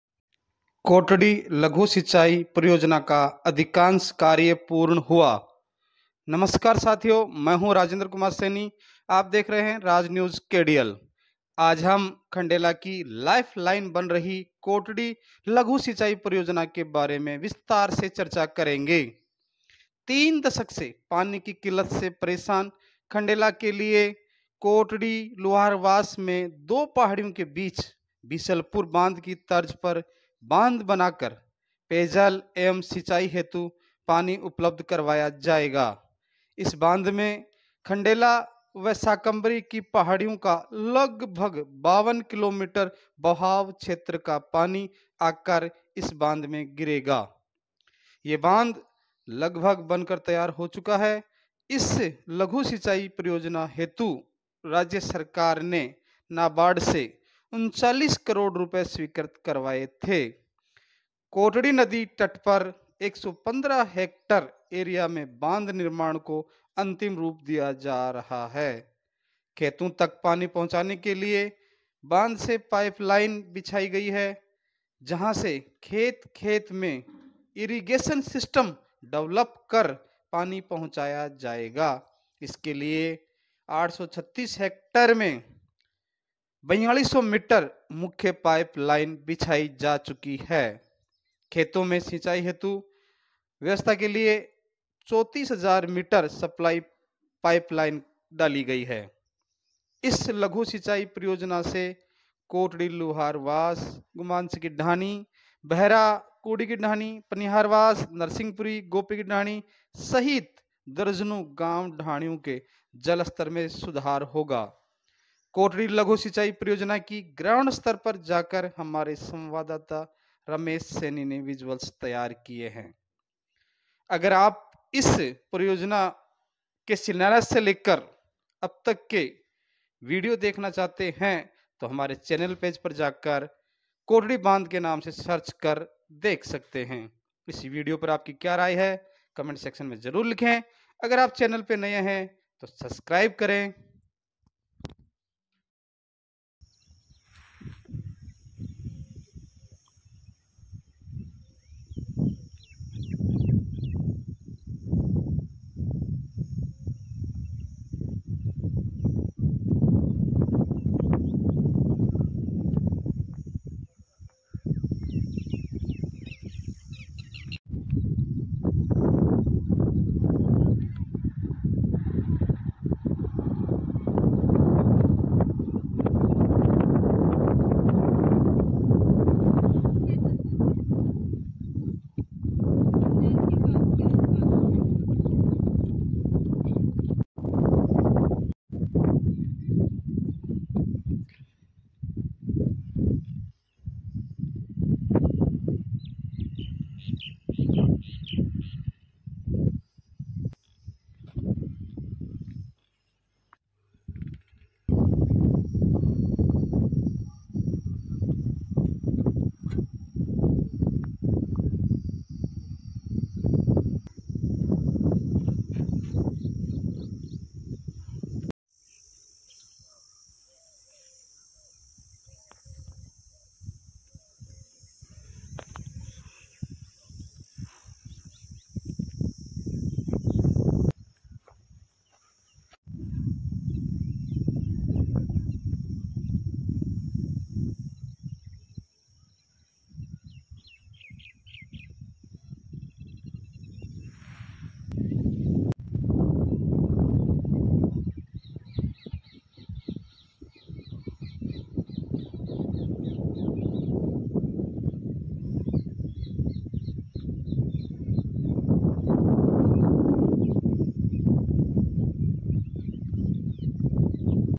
पहाड़ियों के बीच रुकेगा कोटडी नदी का पानी ~RajNewsKdl
दो पहाड़ियों के बीच कोटडी नदी पर बीसलपुर बांध की तर्ज पर चौथा सबसे बड़ा बांध ~RajNewsKdl